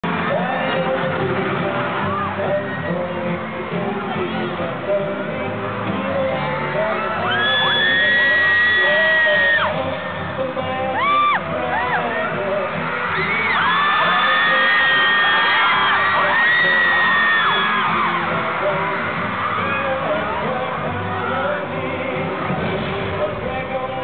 [100410] Supershow 2 Manila - Ryeowook butt dance
he's so goodlooking and sexy omo..but he seems sick..keeps on coughing at the start of concert =(